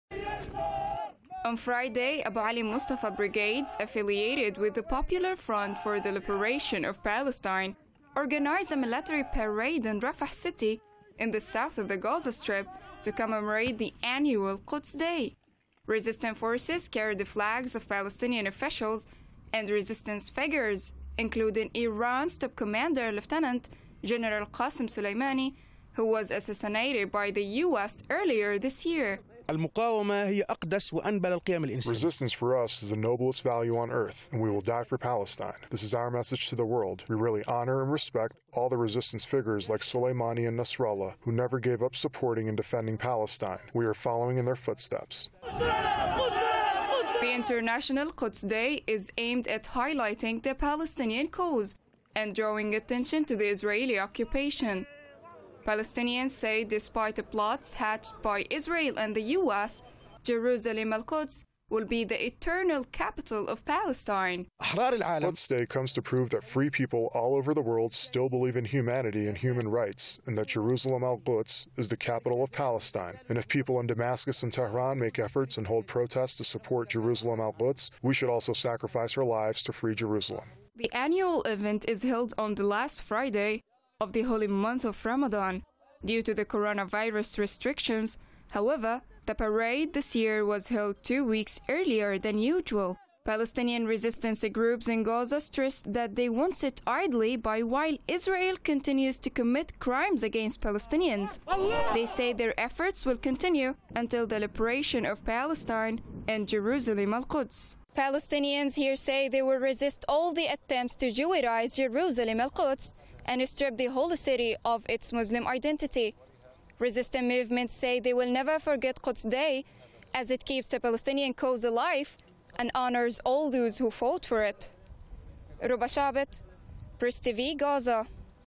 [AlQuds2020] Military parade held in Gaza to commemorate Quds Day
On Friday, Abu Ali Mustafa brigades, affiliated with the Popular Front for the Liberation of Palestine, organized a military parade in Rafah city in the south of the Gaza Strip to commemorate the annual Quds Day.

Resistance forces carried flags of Palestinian officials and resistance figures, including Iran's top commander Lieutenant General Qassem Soleimani, who was assassinated by the US earlier this year.

The International Quds Day is aimed at highlighting the Palestinian cause and drawing attention to the Israeli occupation. Palestinians say despite plots hatched by Israel and the US, Jerusalem al-Quds will be the eternal capital of Palestine.

The annual event is held on the last Friday of the Holy Month of Ramadan. Due to the coronavirus restrictions, however, the parade this year was held two weeks earlier than usual.

Palestinian resistance groups in Gaza stress that they will not sit idly by while Israel continues to commit crimes against Palestinians. They say their efforts will continue until the liberation of Palestine and Jerusalem al-Quds.

Palestinians here say they will resist all the ongoing attempts to Judaize Jerusalem al-Quds and strip the holy city of its Muslim identity. Resistance movements say they will never forget Quds day as it keeps the Palestinian cause alive and honors all those who fought for it.